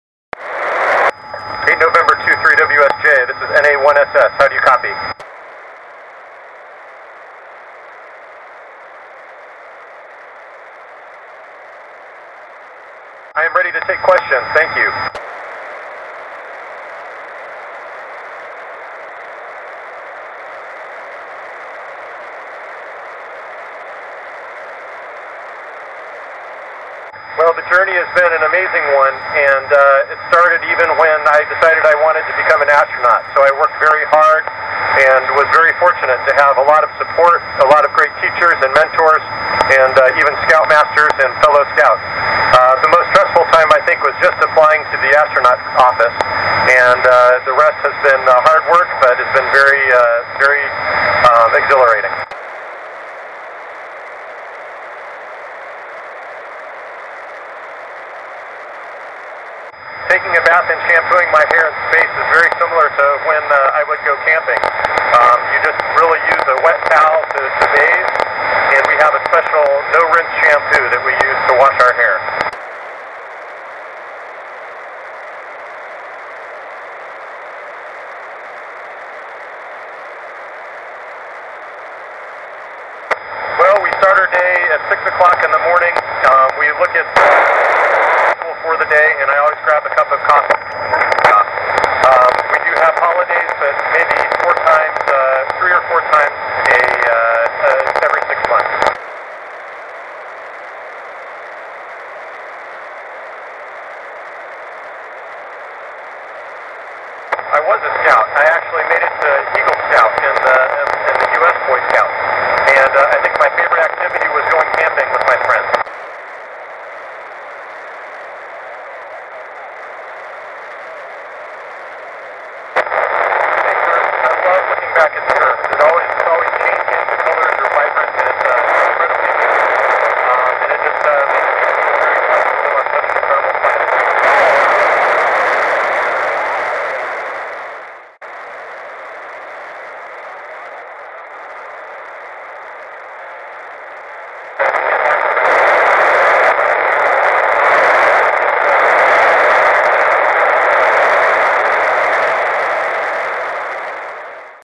International Space Station ISS to 23rd World Scout Jamboree Nippon Boy Scout Amateur Radio Club
International Space Station ( ISS ) school contact to 23rd World Scout Jamboree Nippon Boy Scout Amateur Radio Club, Bunkyo-ku, Japan, direct via 8N23WSJ. The ISS callsign is presently  scheduled to be NA1SS. The scheduled astronaut is Kjell Lindgren  KO5MOS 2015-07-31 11:26:54 UTC. VHF Radio downlink recorded by BX2ADJ Y.C. Shiau, MD, PhD, Taipei, Taiwan.
ISS 國際太空站 與學校通話: 第23屆世界童軍大露營 業餘無線電社，日本東京都文京區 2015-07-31 中原時 19:29 VHF無線電下鏈 錄音: BX2ADJ 蕭醫師，新北市.
73